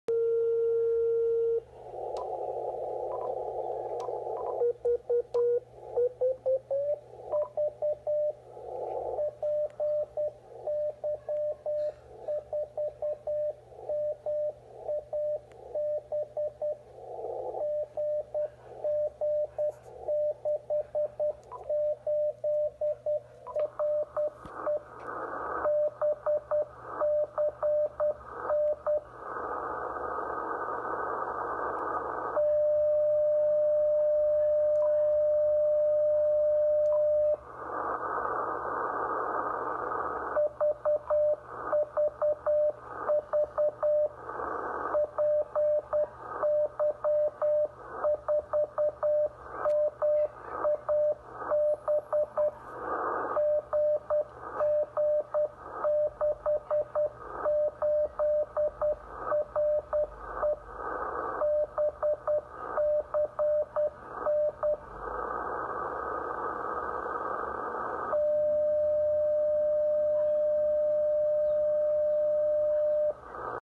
BEACON 28,270.6
Beacon 24h 10 watts antena vertical. Iniciou suas transmissões em Dezembro de 2010